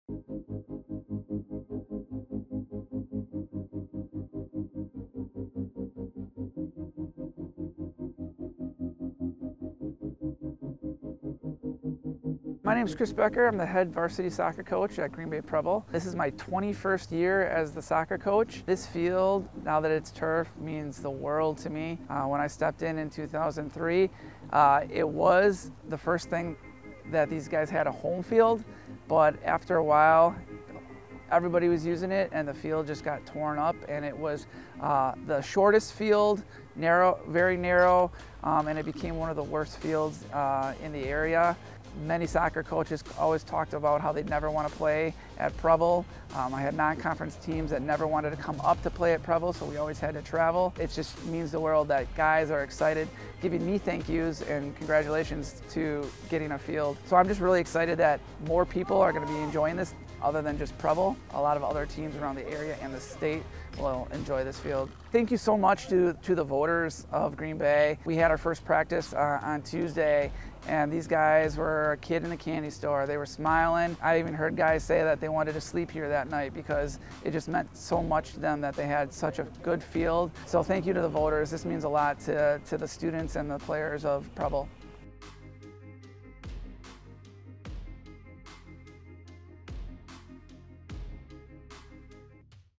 Thank You Green Bay: Preble High School Soccer Coach on 2022 Referendum
Preble High School varsity soccer coach Chris Becker wants to thank the greater Green Bay community for supporting the 2022 Referendum. Preble Soccer made its home debut on the new turf field at Preble High School on September 14, 2023. Preble beat Green Bay Southwest High School 7-0. ⚽

Head here to learn more about the referendum projects, both in-progress and projects to come